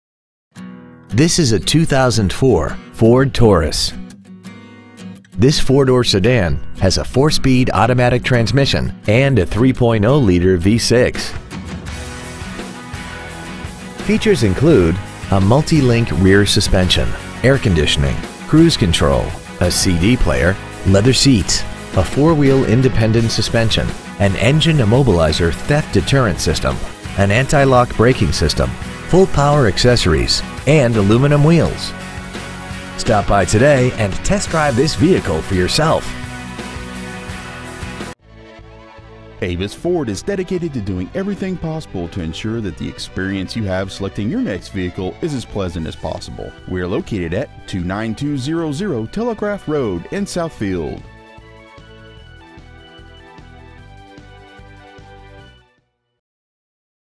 2004 FORD TAURUS Southfield MI
We have been honored to serve the Southfield MI area , we promise that your experience at our dealership will exceed your expectations !

 Year : 2004
 Make : FORD
 Model : TAURUS
 Engine : 3.0L V6 24V MPFI DOHC
 Trans . : 4-SPEED AUTOMATIC
 Exterior : MERLOT CLEARCOAT METALLIC
 Miles : 96,753
 Interior : MEDIUM PARCHMENT
 Stock : 187636